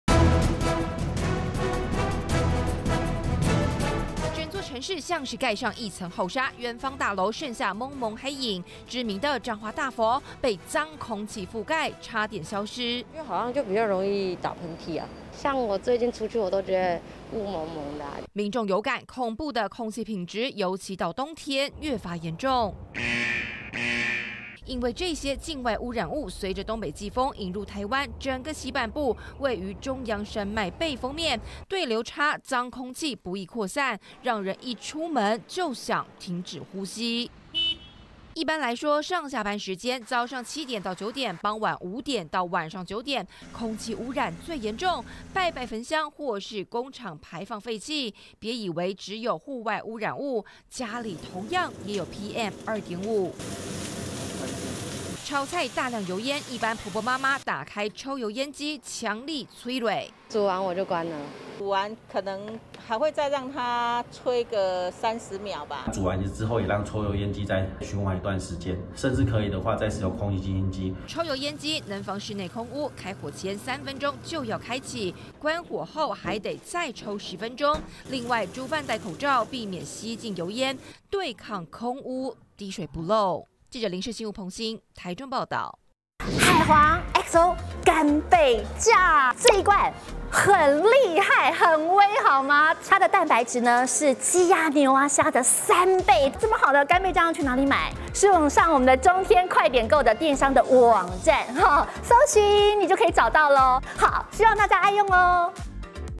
【每日必看】冬天空汙比夏天嚴重 空氣"最髒時間"曝光 20221115@中天新聞CtiNews
這幾天中南部空氣品質拉警報！尤其冬天空汙又比夏天來的更嚴重，主要因為東北季風帶來境外汙染物，對流不易，無法吹散髒空氣，另外每到上下班時間車輛排放廢氣，早上七點到九點、傍晚五點到晚上九點汙染最嚴重！不只戶外，家中也有PM2.5，料理時製造油煙，專家建議抽油煙機在開火前三分鐘開啟，關火後再開十分鐘，避免人體吸入過多油煙。